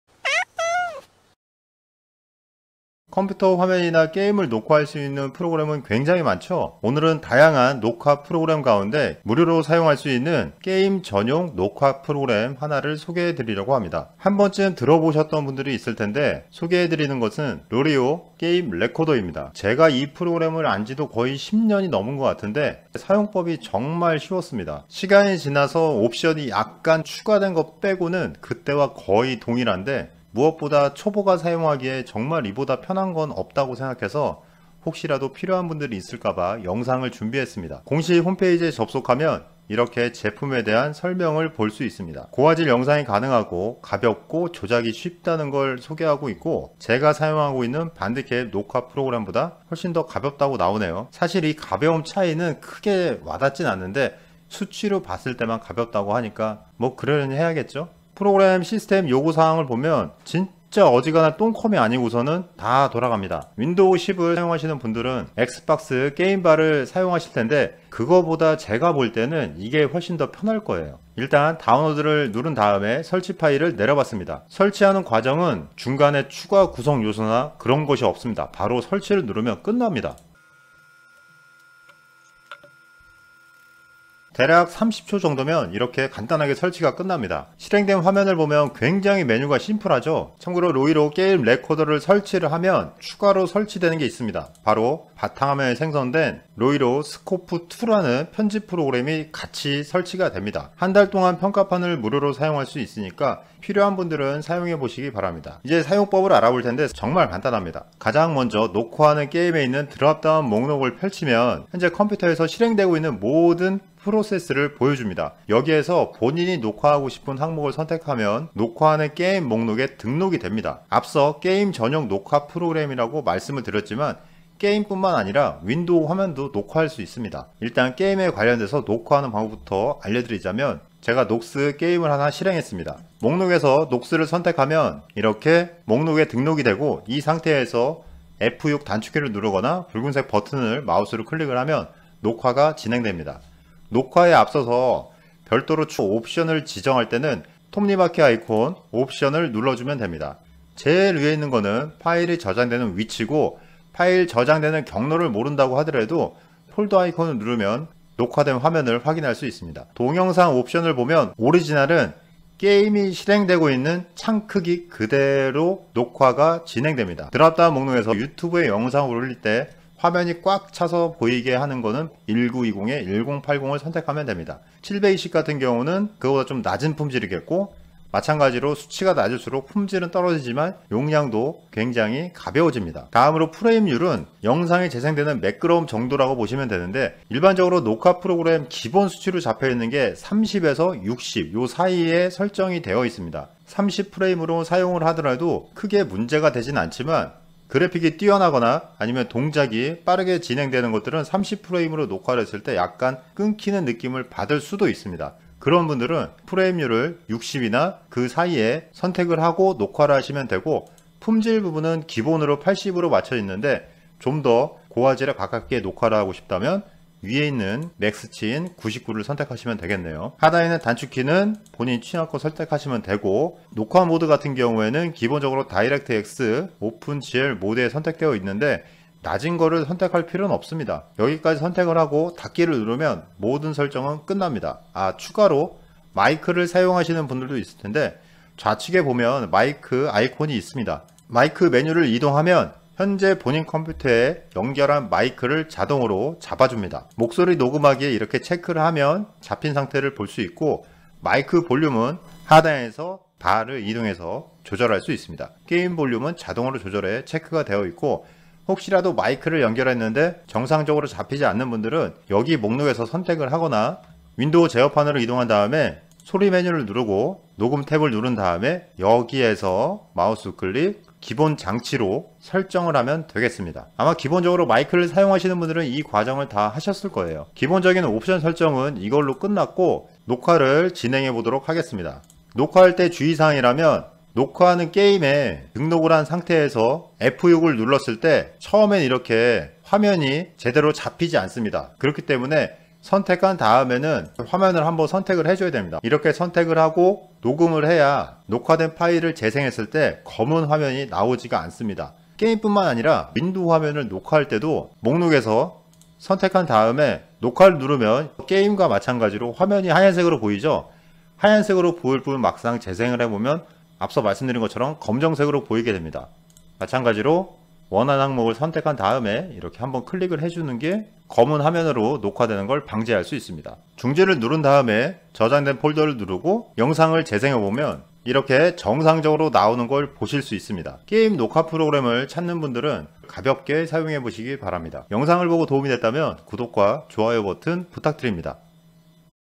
심플한 컴퓨터 게임 화면 무료 녹화 프로그램 추천! loilo game recorder 다운로드 및 자세한 사용법
loilo game recorder는 게임 뿐만 아니라 컴퓨터 화면 녹화도 가능합니다.
복잡한 옵션 설정도 없고 사용법이 쉬워 녹화 프로그램 사용에 서툰 분들에게 추천합니다.
예전부터 계속 무료로 제공하고 있고 앞으로 계속 무료로 이용할 수 있습니다.

영상에도 소개했지만 검은 화면으로 녹화되는 부분만 주의하면 어렵지 않습니다.
영상을 보고 도움이 됐다면 구독! 좋아요! 부탁드립니다.